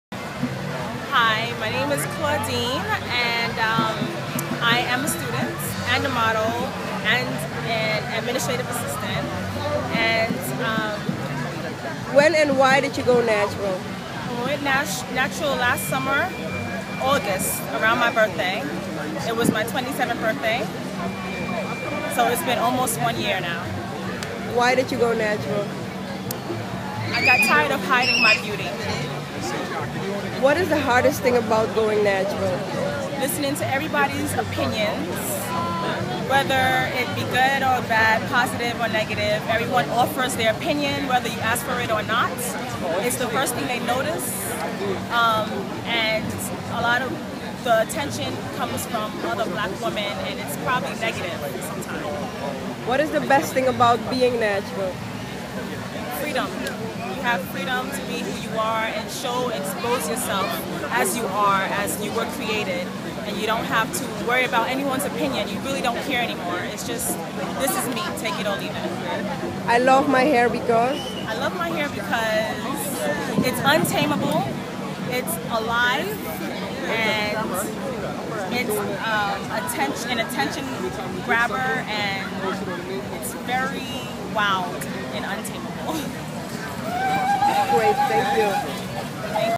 Claudine's Natural Hair Story
Claudine shares her hair story with Going-Natural.com at the African Arts Festival.